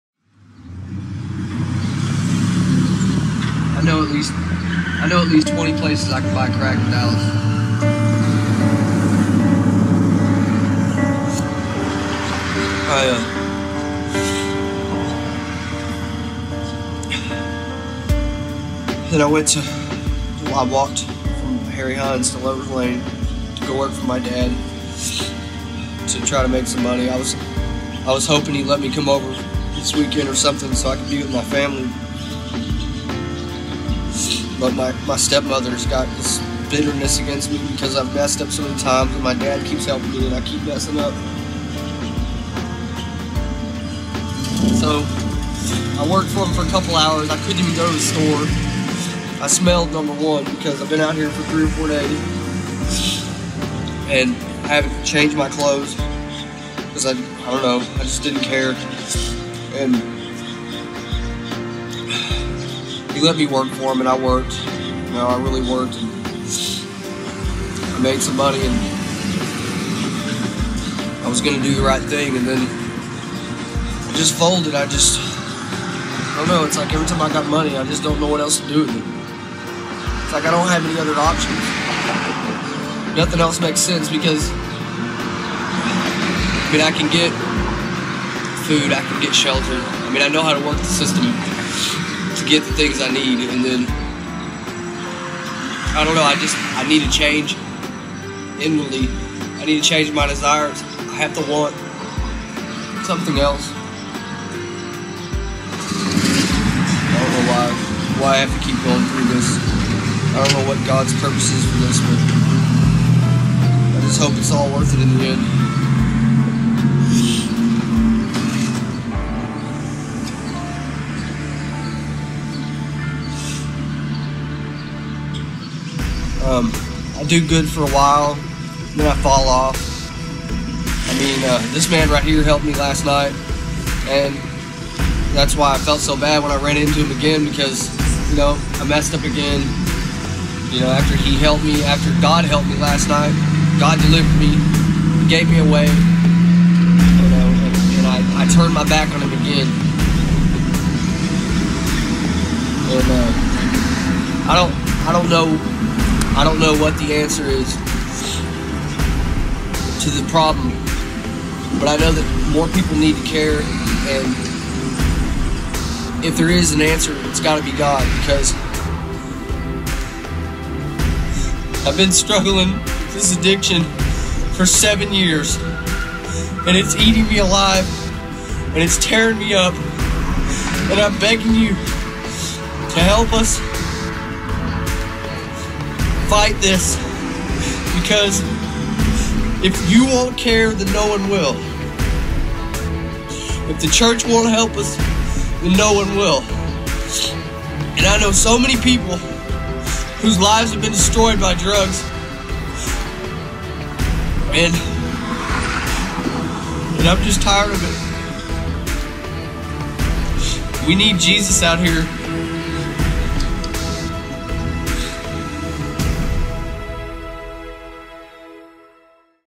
Homeless Plea
a homeless guy shares his story about addiction, family and his desire for change